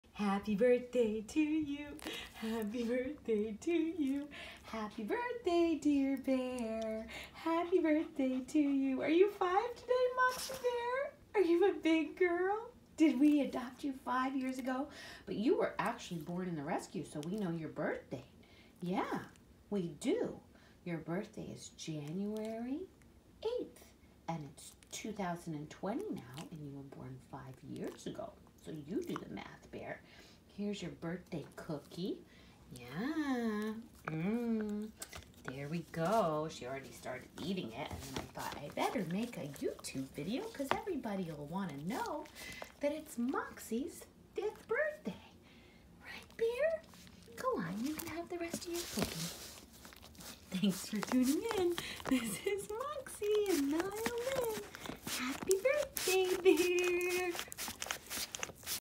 Moxy turns 5 - Shih Tzu Rescue celebrating her birthday.
Happy Birthday, Bear! This is a special cookie made for dogs that I got from the pet store.